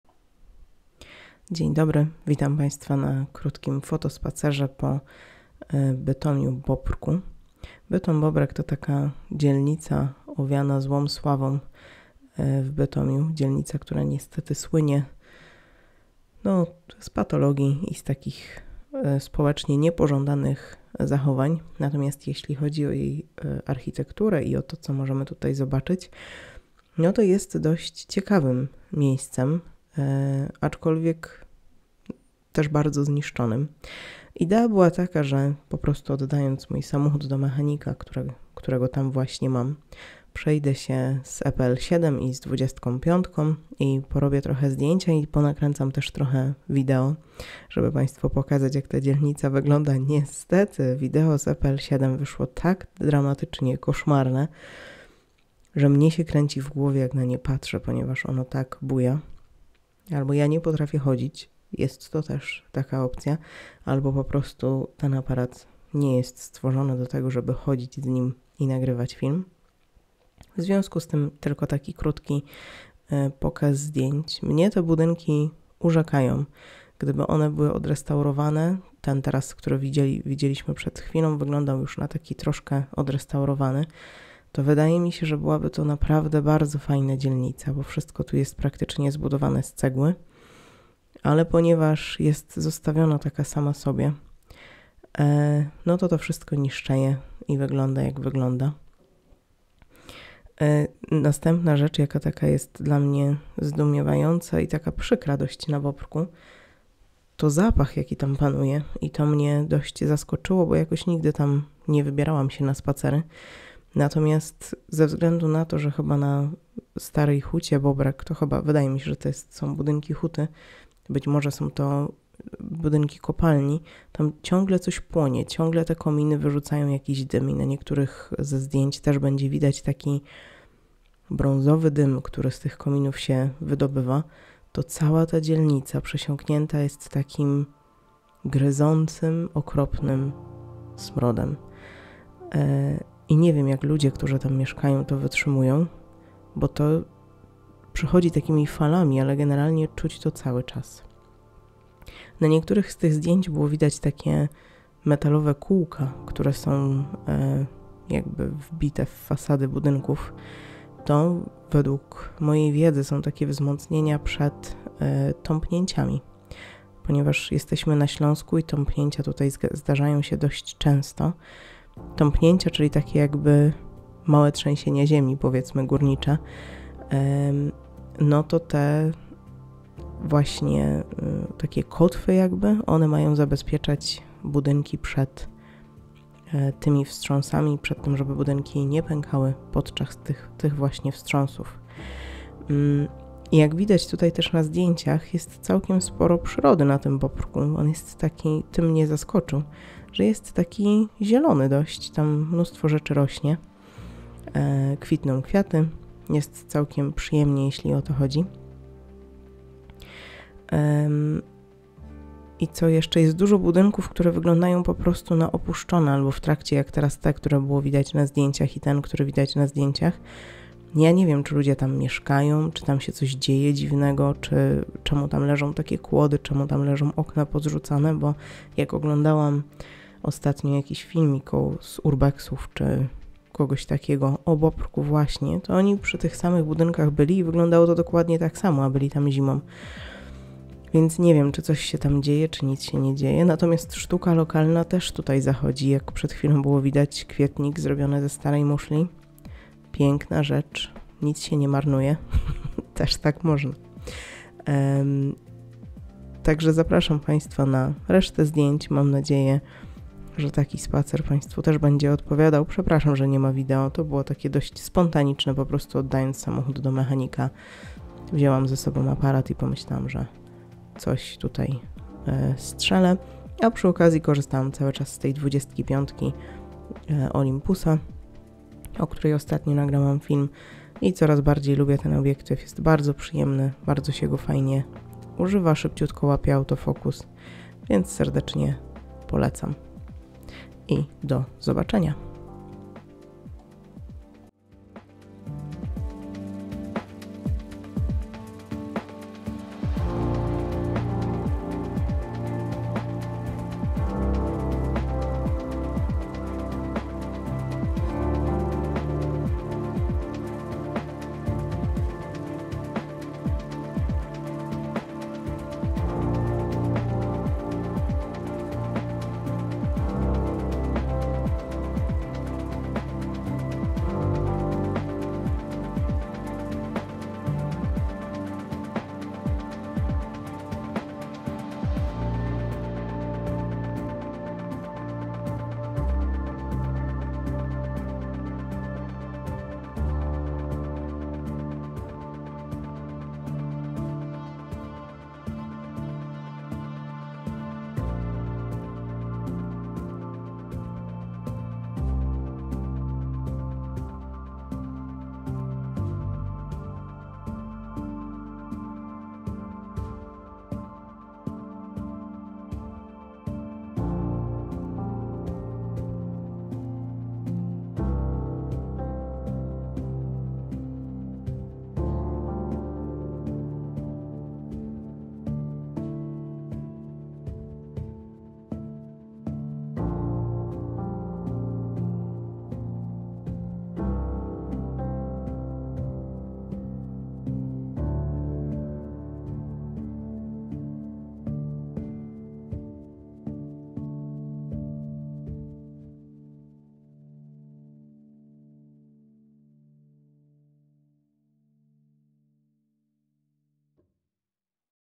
Bytom Bobrek na zdjęciach - foto spacer z Olympus e-pl7 i 25mm f1.8
Zapraszam na fotospacer po okrytej złą sławą dzielnicy Bytomia - Bobrek